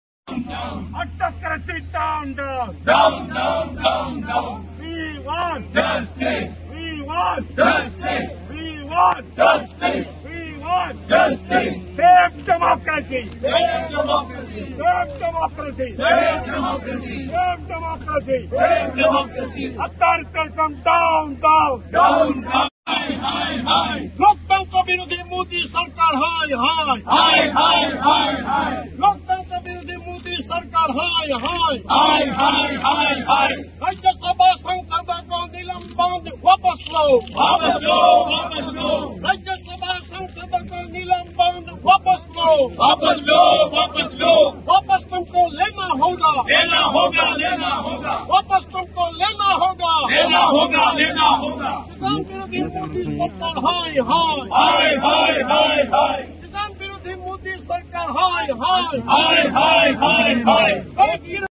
Shri Rahul Gandhi & MPs of like minded opposition parties protesting at Gandhi statue in Parliament
Shri Rahul Gandhi & MPs of like minded opposition parties protesting at Gandhi statue in Parliament House